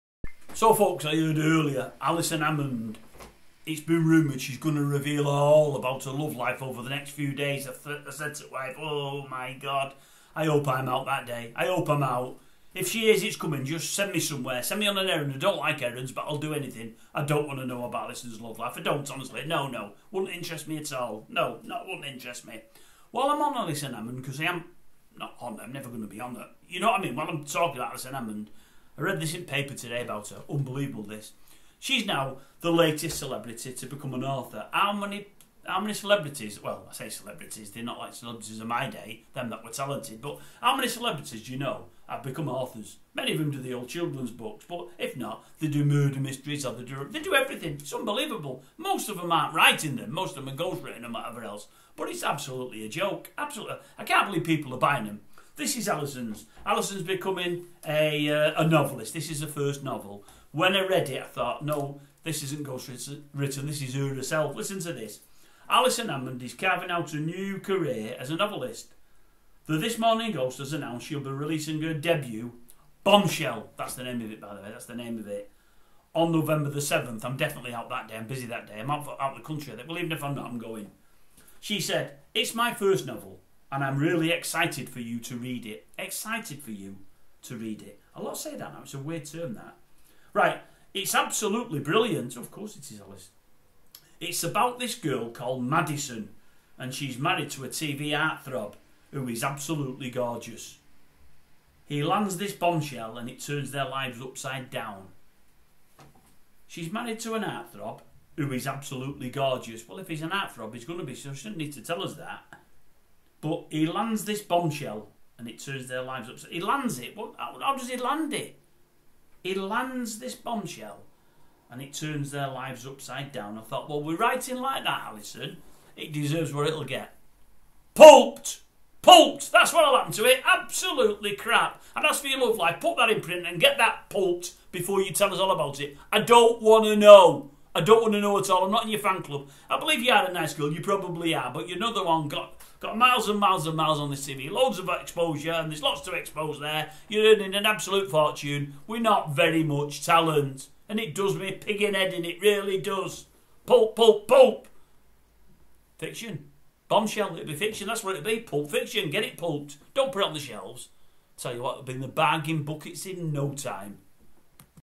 ALISON HAMMOND to tell all ABOUT HER LOVE LIFE - why says I wait a minute
has she got a book out ?   YES SHE HAS   well what a BLOODY SHOCK